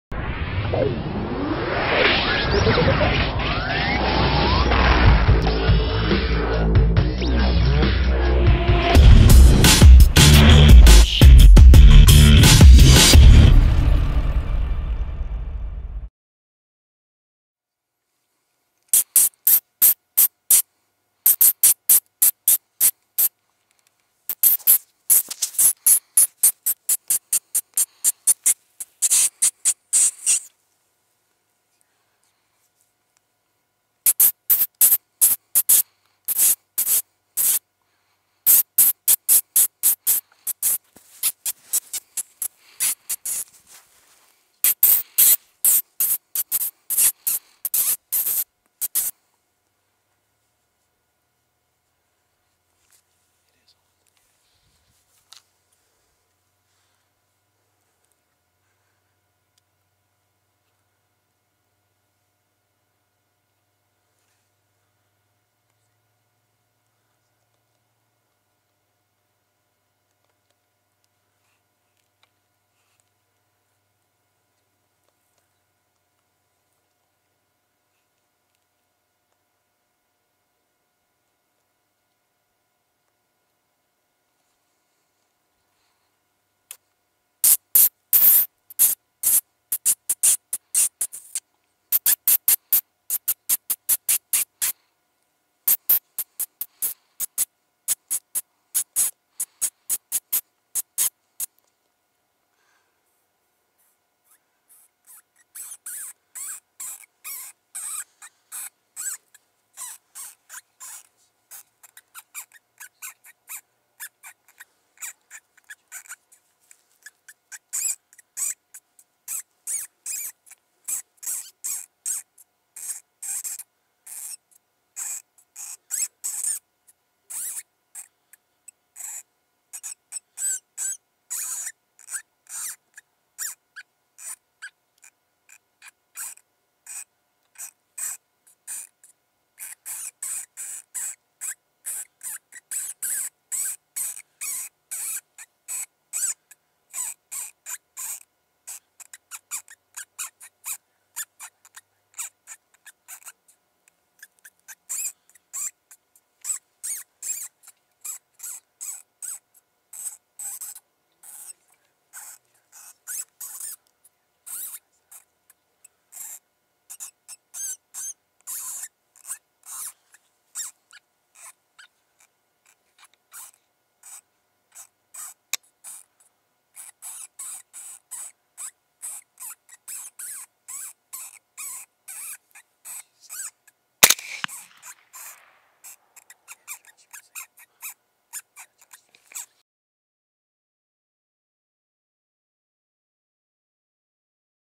ATN X Sight II 5-20 Headshot
ATN X Sight II 5-20 shows this coyote all the way in and gives me the change to place the bullet right where I wanted it!